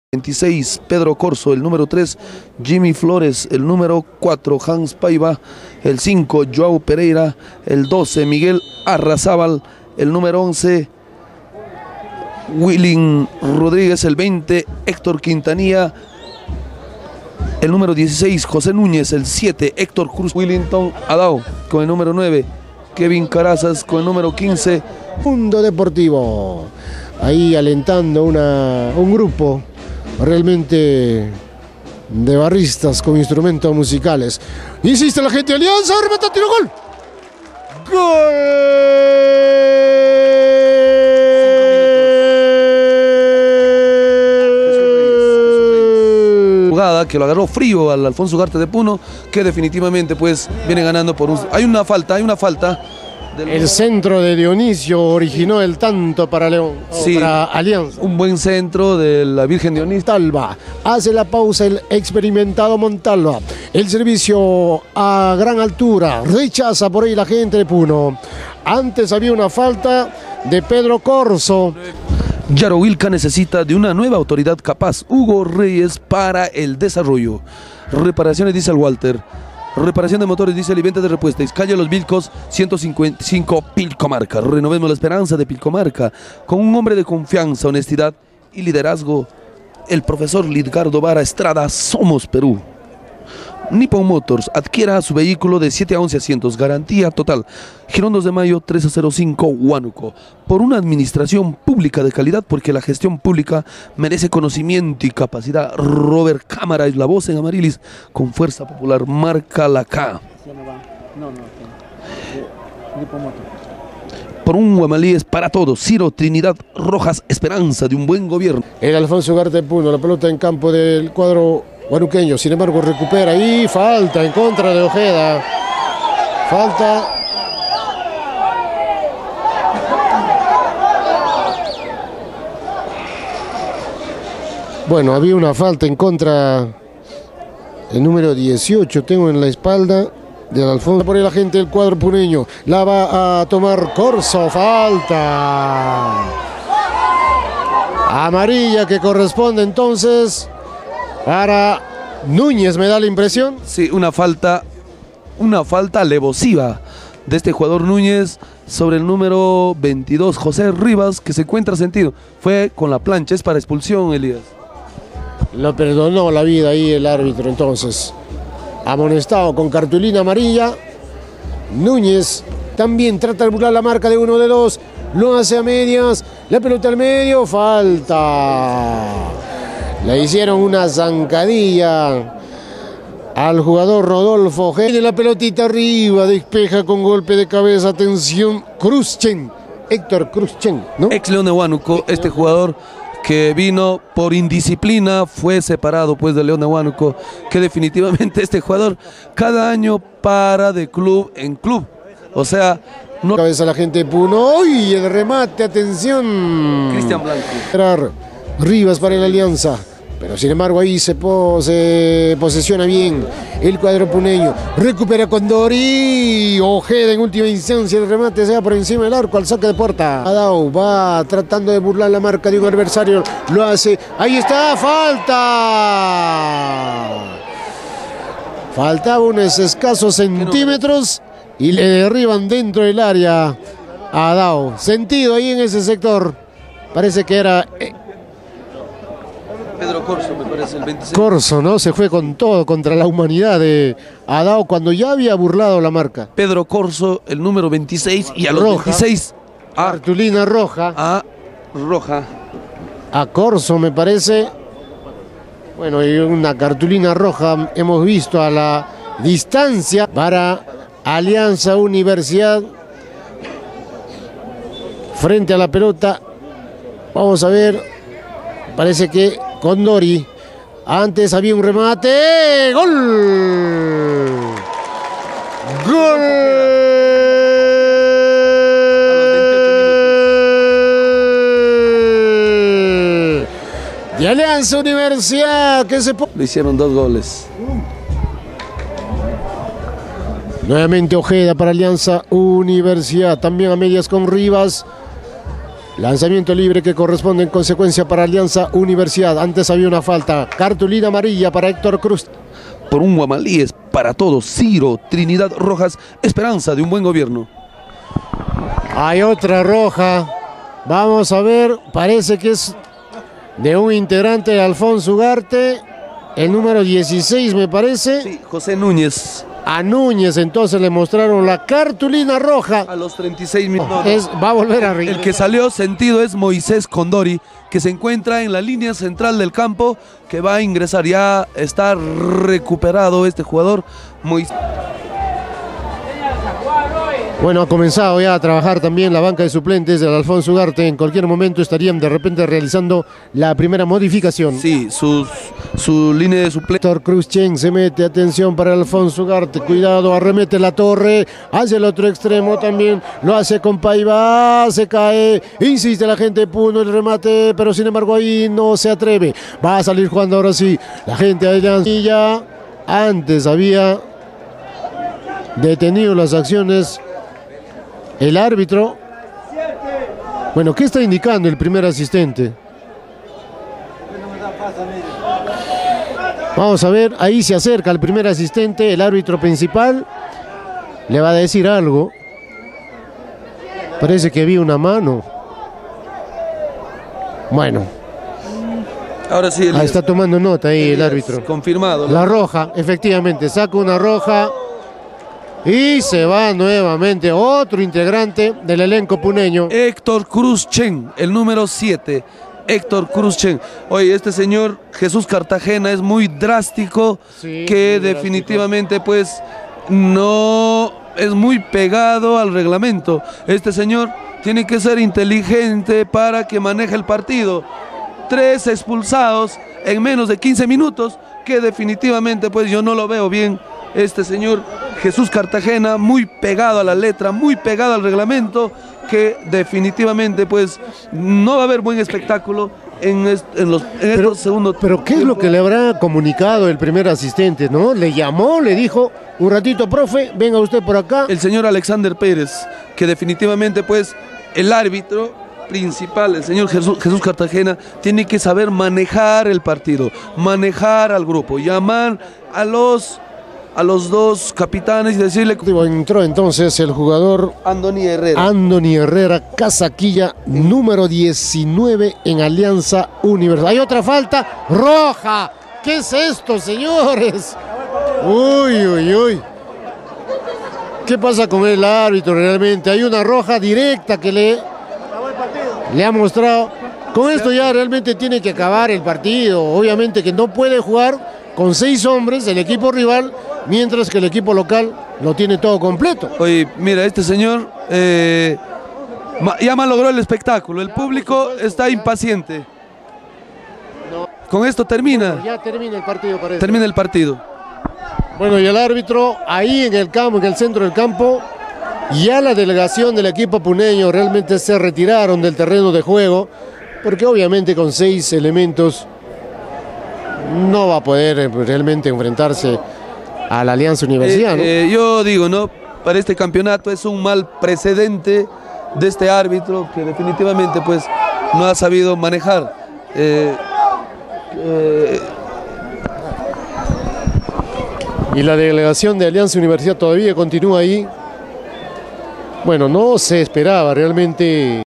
Alianza Universidad 2-0 Alfonso Ugarte
Goles y detalles del partido de Alianza Universidad - Alfonso Ugarte y de la forma vergonzosa de como termino el partido, la falta de respeto al publico por parte de la visita y la actuación pésima del arbitro que permitió que el partido se le vaya de las manos.